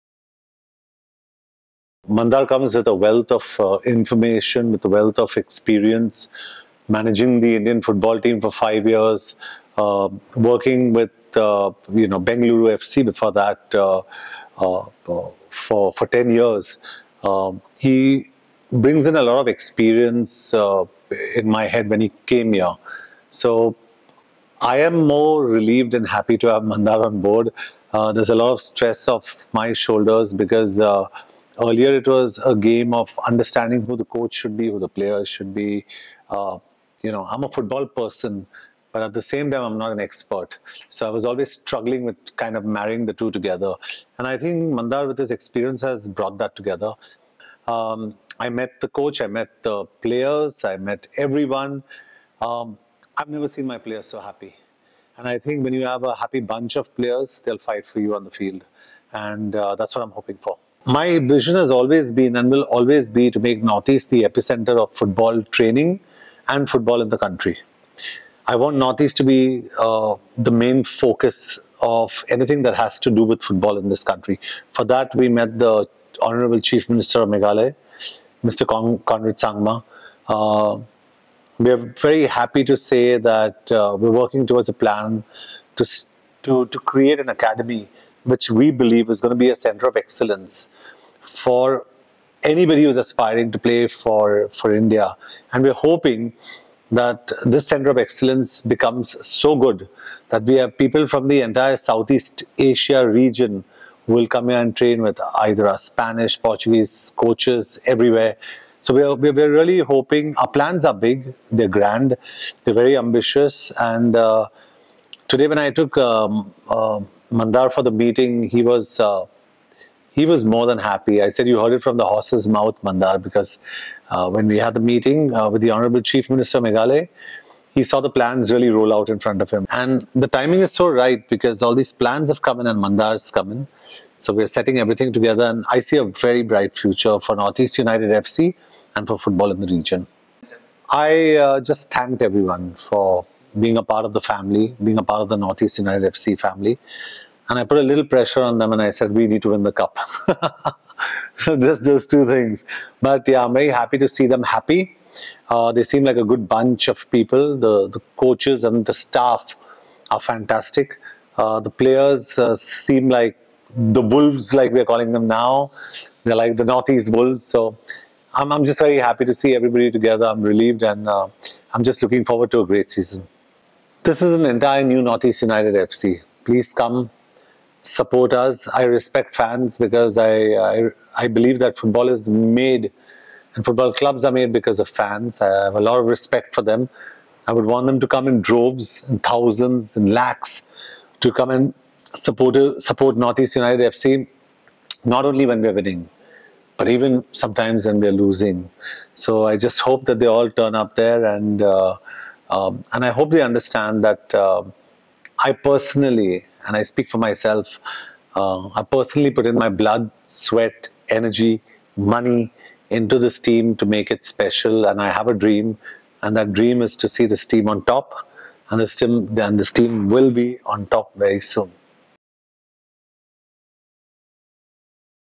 John Abraham Interview | NorthEast United FC
🗣️ "I see a very bright future for NEUFC & football in the region."

Owner John Abraham shares his vision for the club's exciting journey under Mandar Tamhane and sends a heartfelt message to our dedicated fans ⚽🌟